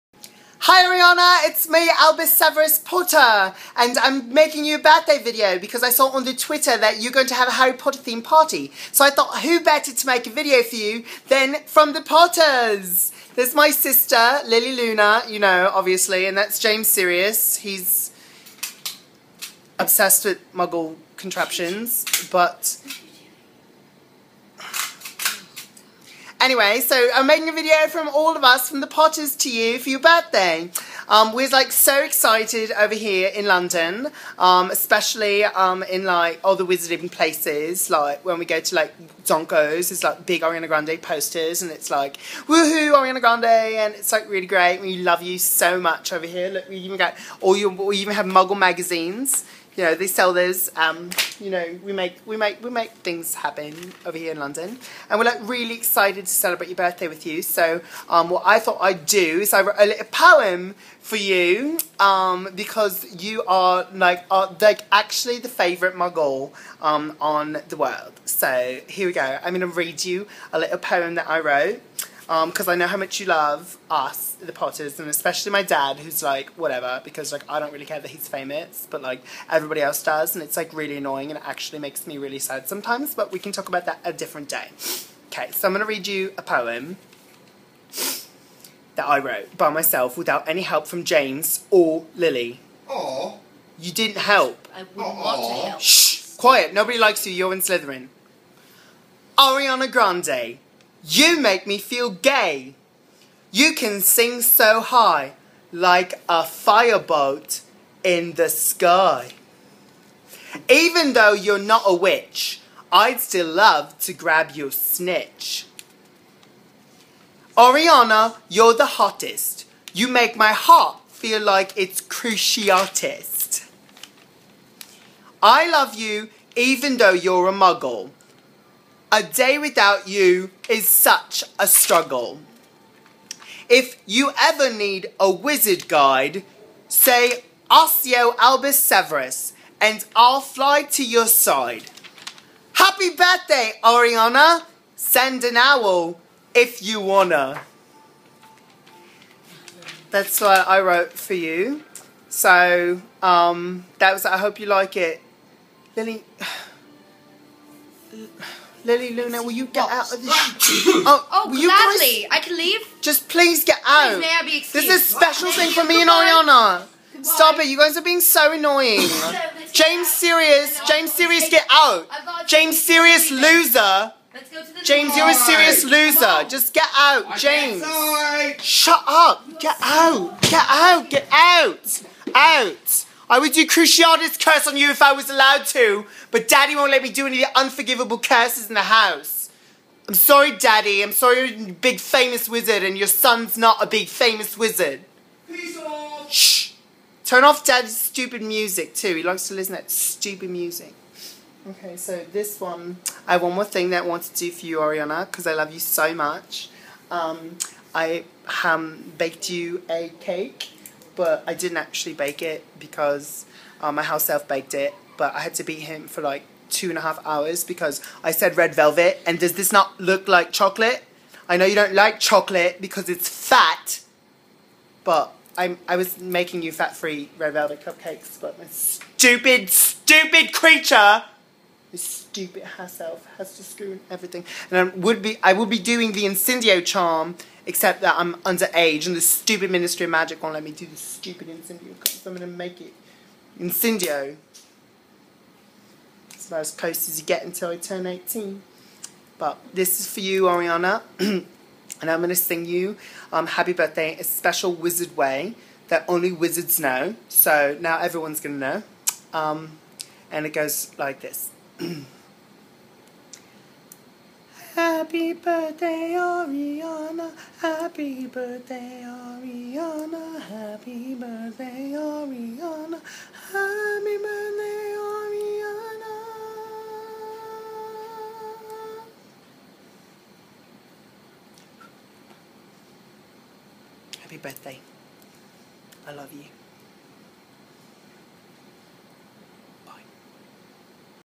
HAPPY BIRTHDAY ARIANA GRANDE FROM THE POTTERS!
Hi everyone! I'm Albus Severus Potter and I love all things Ariana Grande & Australia! I really hope you like this video Ariana... I worked REALLY HARD ON IT!